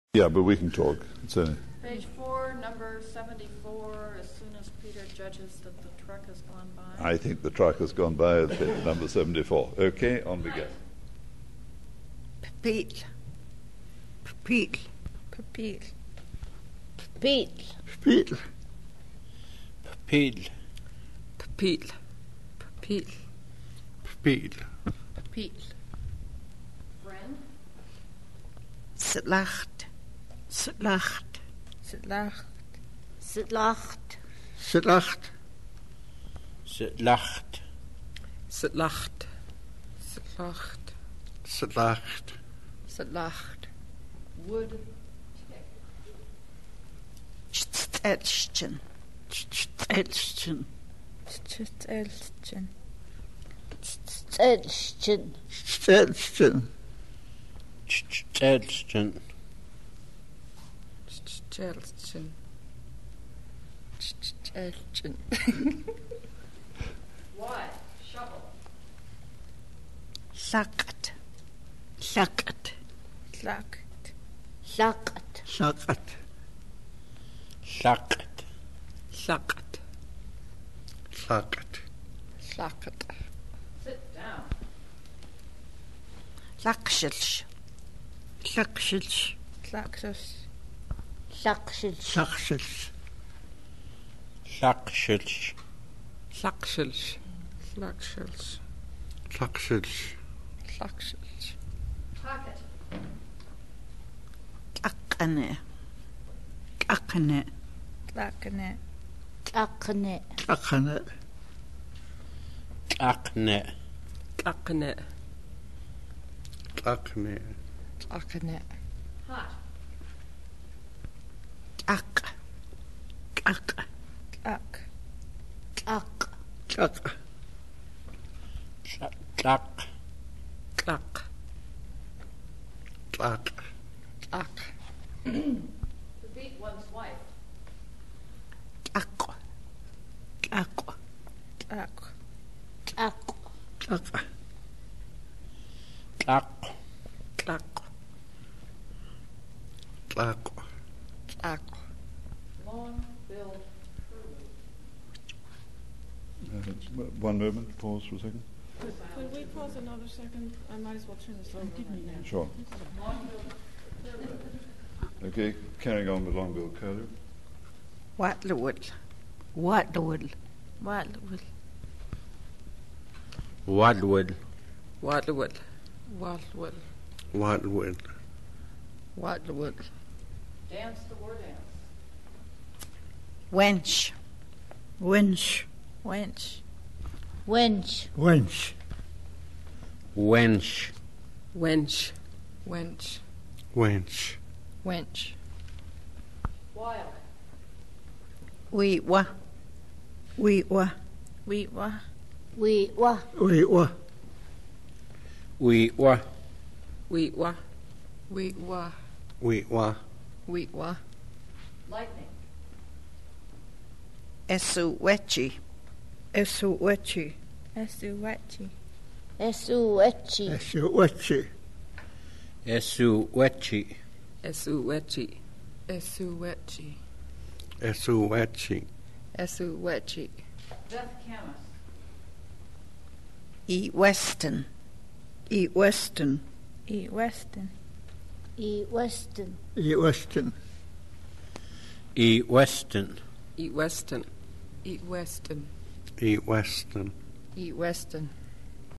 Flathead audio: fla_word-list_1992_30
Flathead (fla) audio: fla_word-list_1992_30
Language family: Salish; Interior Salish; Southern;
Flathead Cultural Center, Flathead Reservation, Montana
Date: 4/5/1992

Description: Language Montana Salish (Flathead).  fla_word-list_1992_01.html, enties: 70 - 82

Contributor: Harriet Whitworth, Felicite McDonald, Melanie Sandoval, Joe Eneas, Dorothy Felsman, Antoni (Tony) Incashola, Lucy Vanderburg, Marie Torosian, Clarence Woodcock, and Gloria Whitworth